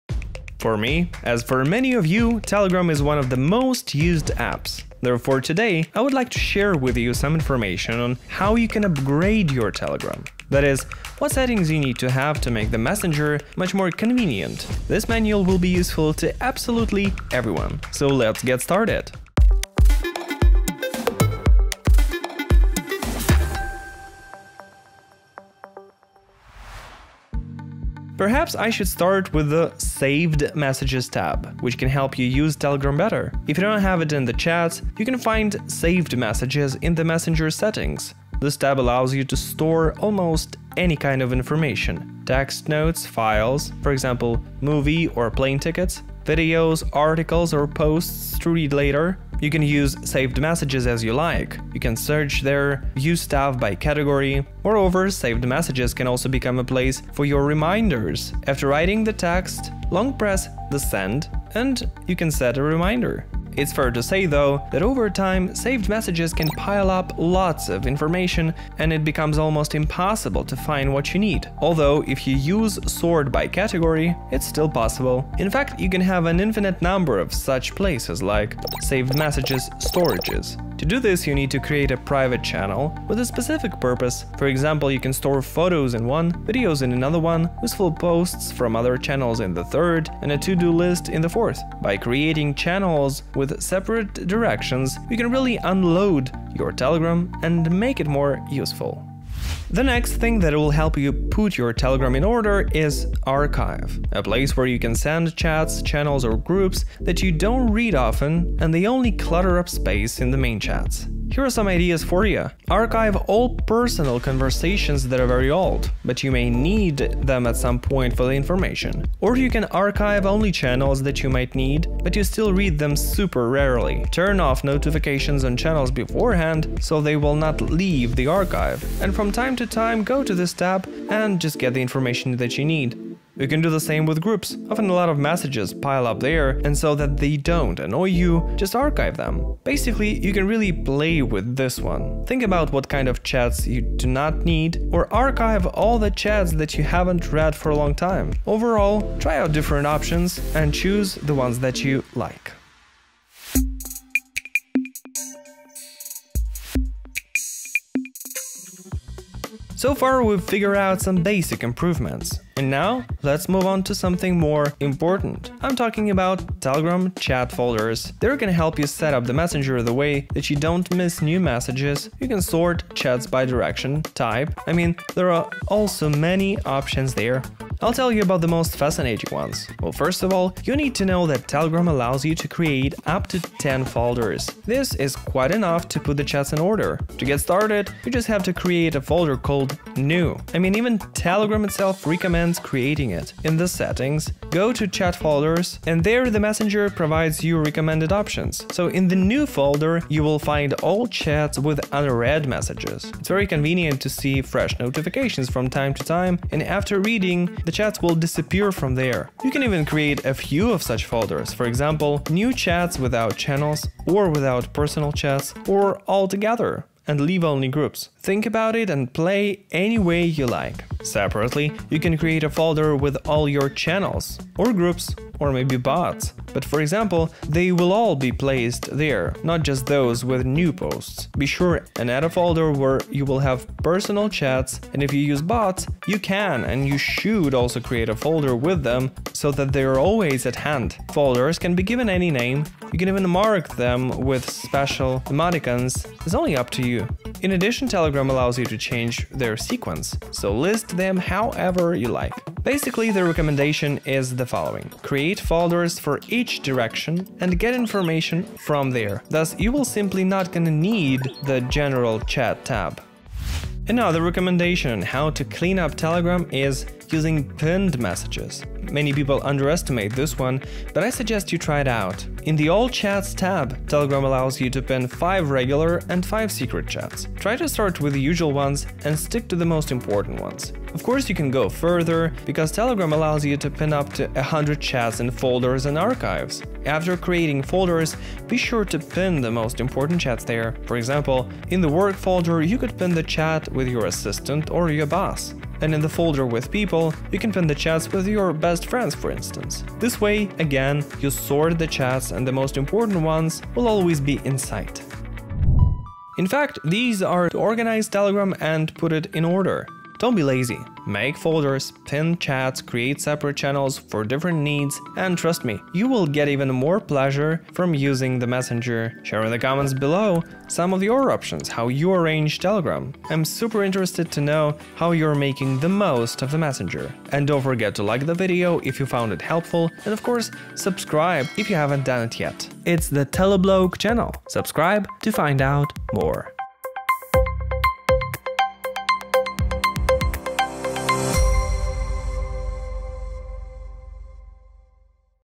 How To Organize Telegram Chats. Perfect Settings
For me, as for many of you, Telegram is one of the most used apps. Therefore, today I want to share with you some information on how to upgrade your Telegram App, that is, what Telegram settings you need to have to make the messenger much more convenient. This manual will be useful to absolutely everyone.

Don't miss:
HOW TO HACK Telegram. Telegram's HIDDEN issues